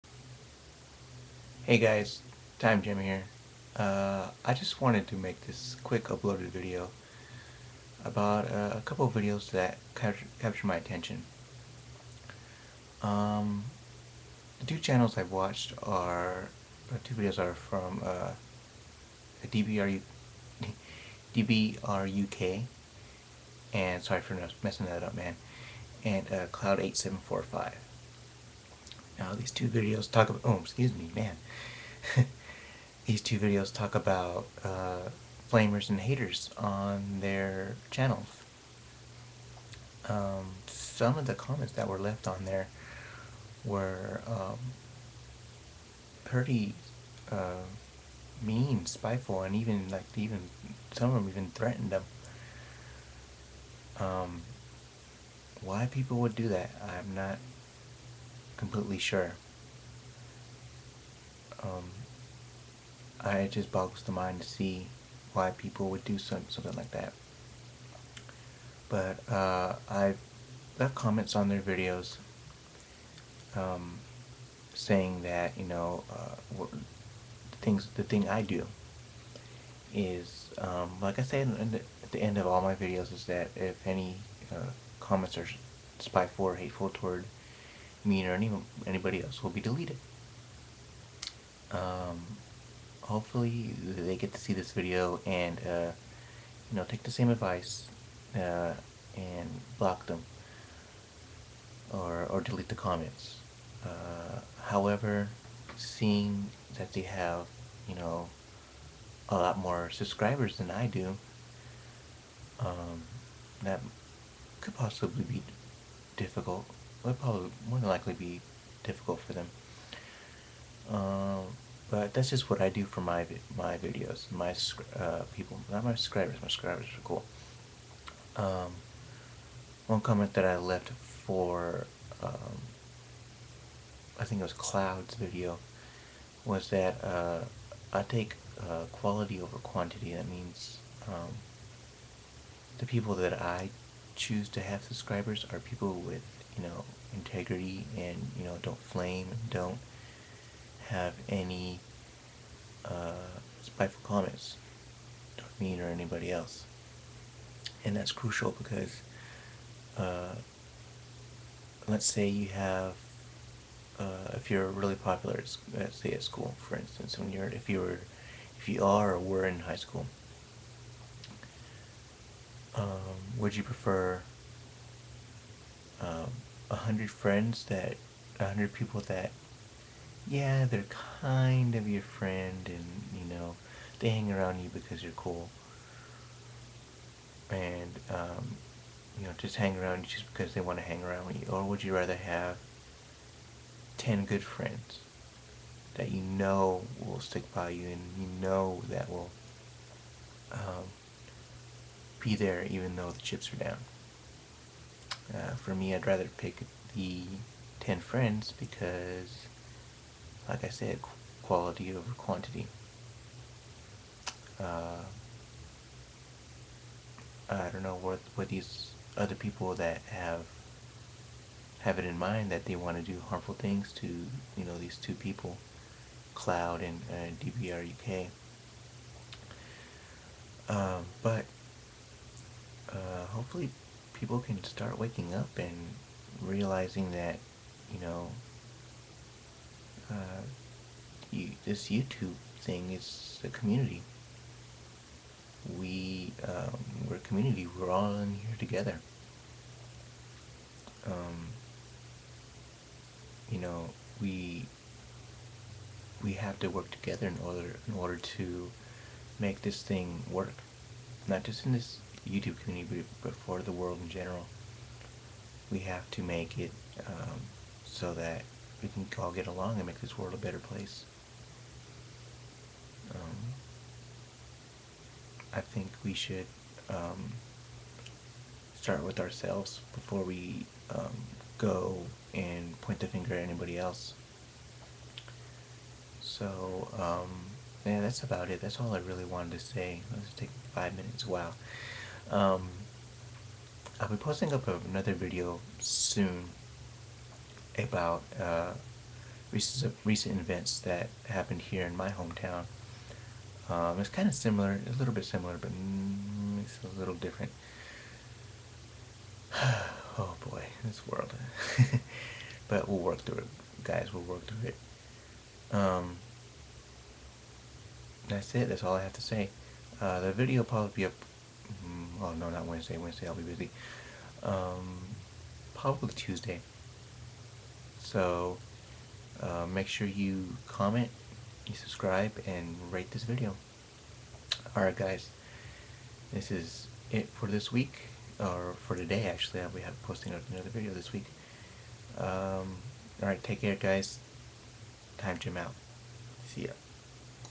Why all the hate? My thoughts on two recent videos
I recently watched two videos about hateful and mean comments that some people are getting on their channel. In this vid, I just share my thoughts on both of these videos.

Hopefully, DBRUK and Cloud8745 will take the right actions and report or block these people from their channels.

Which leads me to my usual ending: No hateful or spiteful comments toward me or anyone else, because they will be deleted.